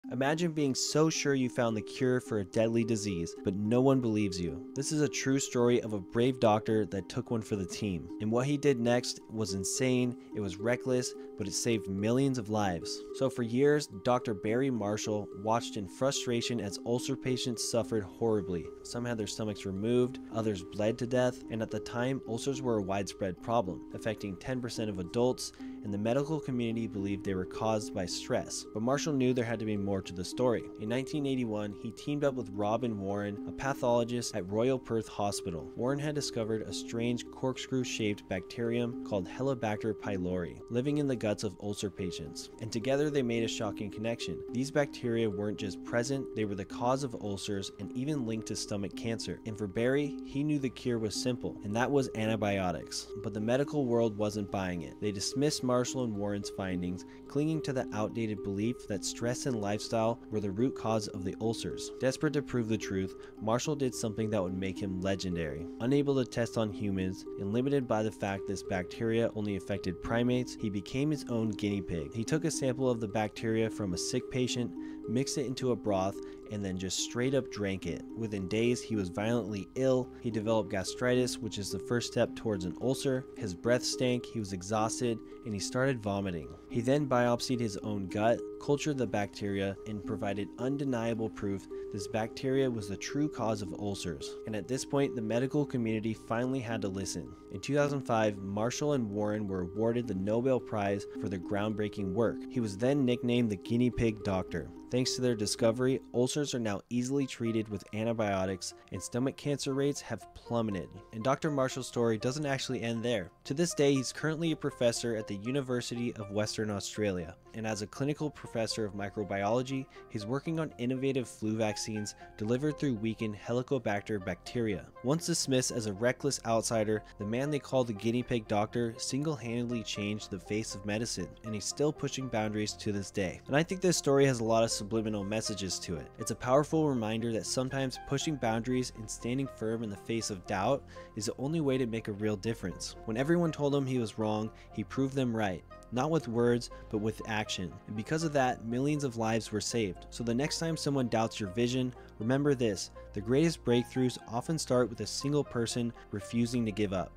The Guinea Pig Doctor: Drank Deadly Bacteria To Prove The World Wrong
Dr. Barry Marshall was called crazy — until he did something unthinkable to prove his life-saving discovery. This is a story of the doctor who risked everything to change medicine forever.